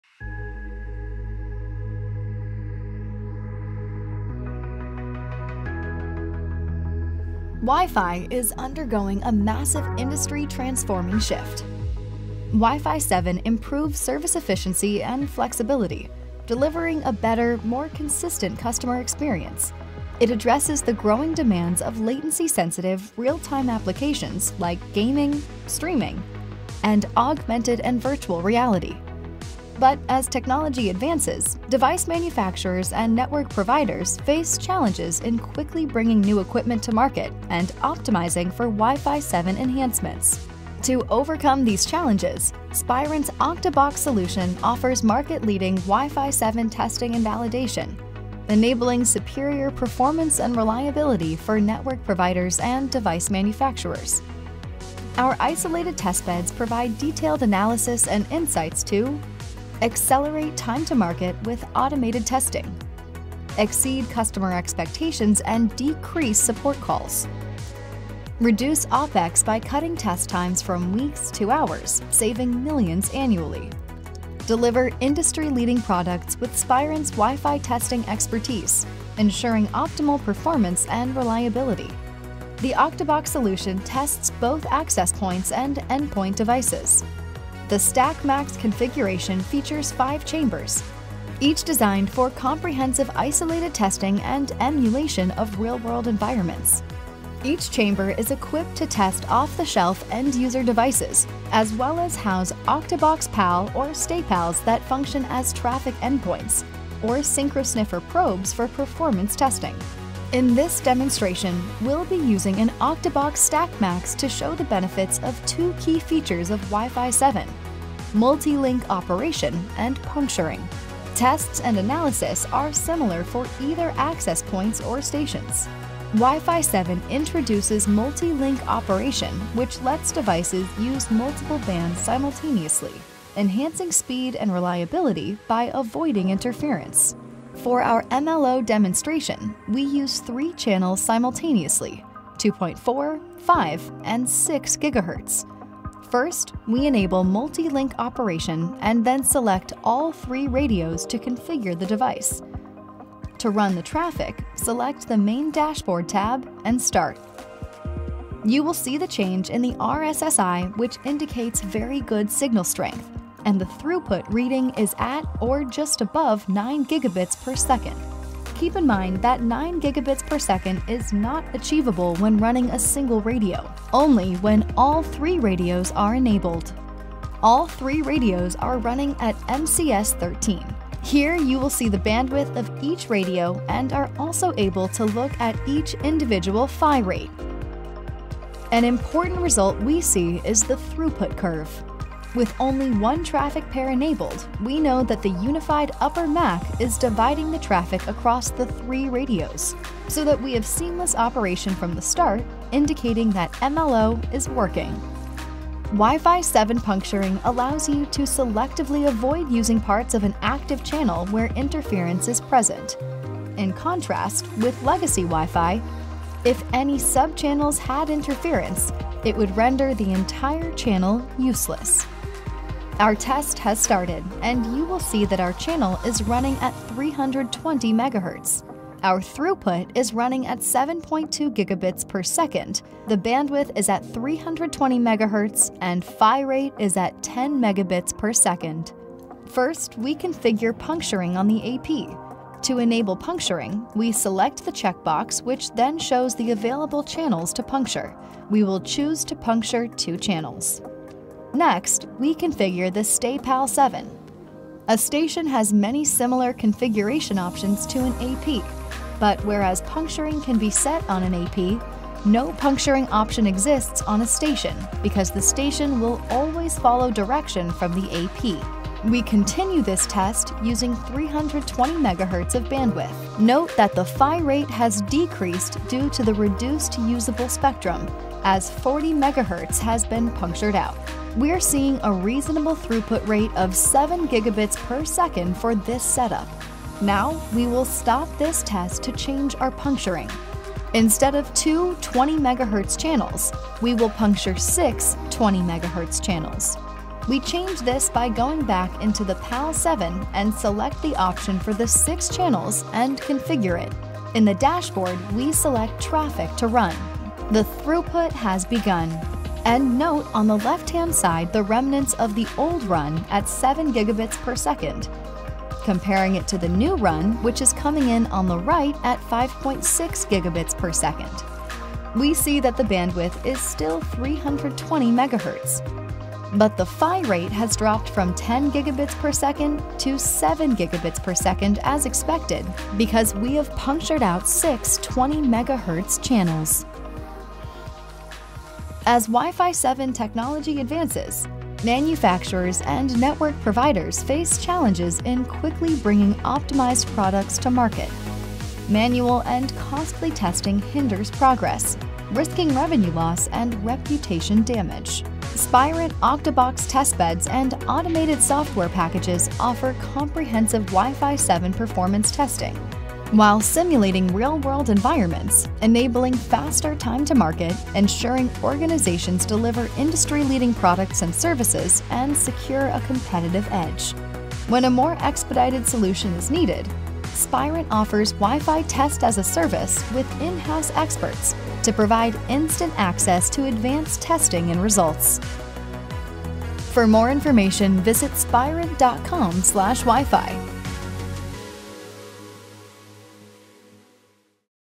Demo - Wi-Fi 7 Network & Device Testing - MLO & Puncturing
Wi-Fi is undergoing a massive, industry-transforming shift, evolving from Wi-Fi 6, Wi-Fi 6E to Wi-Fi 7. With cutting-edge features like Multi-Link Operation (MLO) and Puncturing, Wi-Fi 7 is engineered to handle high-bandwidth, low-latency applications across Wi-Fi networks, from gaming and streaming to VR.

MLO enables Wi-Fi networks to use multiple bands (2.4 GHz, 5 GHz, 6 GHz) at at the same time, maximizing speed, and minimizing interference. Puncturing, meanwhile, lets Wi-Fi devices selectively bypass congested sub-channels, to ensure seamless connectivity in busy environments.

Supporting this shift, Spirent's Octobox Wi-Fi testing platform offers a robust, automated setup for Wi-Fi device testing and validation. This advanced Wi-Fi testing system automates processes, reducing time-to-market and operational costs while enabling manufacturers to deliver top-performing Wi-Fi 7 products.

For immediate needs, Wi-Fi Test-as-a-Service can also provide access to expert testing, ensuring Wi-Fi devices meet high performance standards and competitive demands.

To learn more about these important functions of Wi-Fi testing,

KEY MOMENTS:
00:00 -- Wi-Fi's Big Industry Shift
00:43 -- New Testing Challenges
01:50 -- Key New Features of Wi-Fi
02:07 -- MLO testing in Action
03:26 -- Wi-Fi 7 Puncturing Explained
05:42 -- The Ultimate Wi-Fi Testing Solution